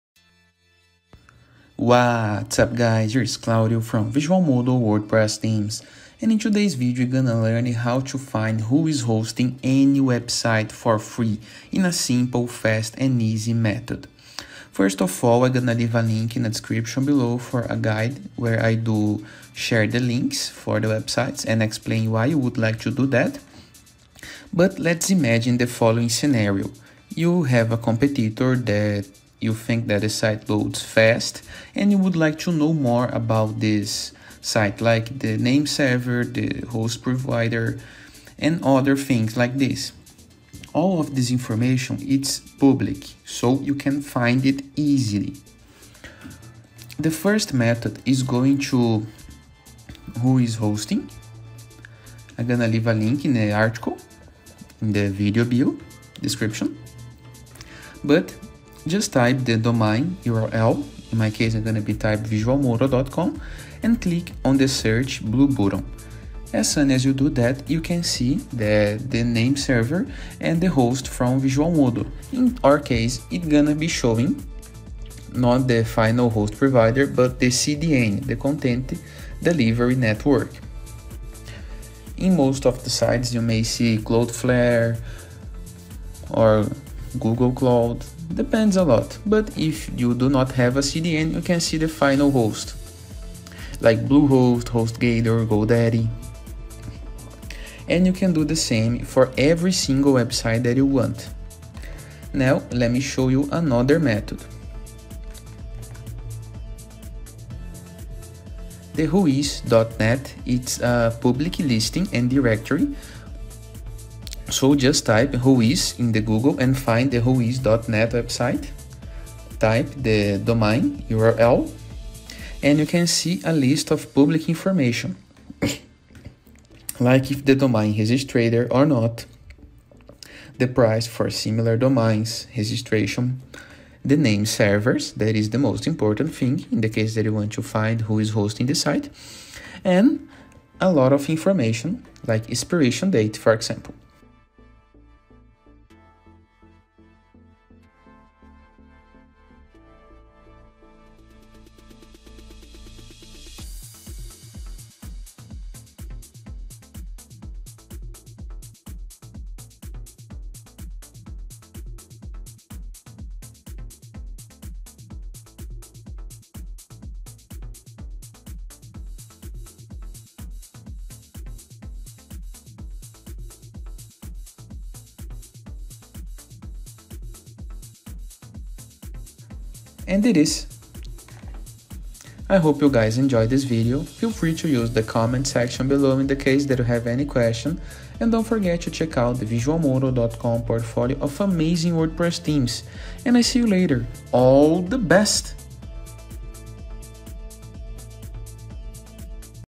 How To Know Who Is Hosting Any Website?
Would you like to see what web hosting company any site is running? This video will explore a fast and straightforward method to know who is hosting a website. It is a ubiquitous question. You probably also want to find out what company is hosting your competitor’s website. This article will show you how to know who is hosting a particular website for free.
I hope you guys enjoy this video, feel free to use the comments section below in the case that you have any questions, and don't forget to check out that Visualmodo website and subscribe to our channel for more web design and development training videos.